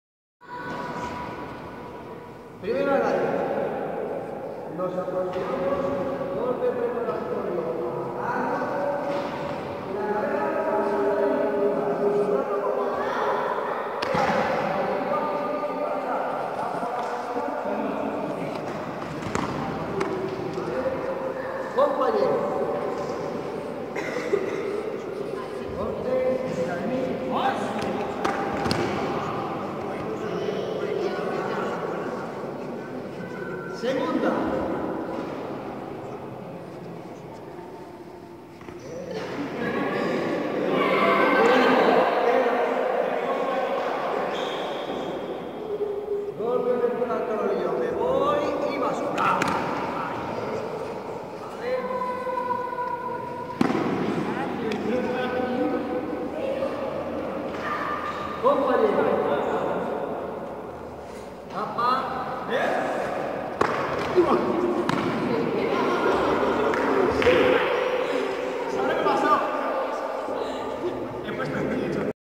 video 2 defensa personal
Felix Vara y sus alumnos en el curso intensivo de defensa personal del mes de Marzo.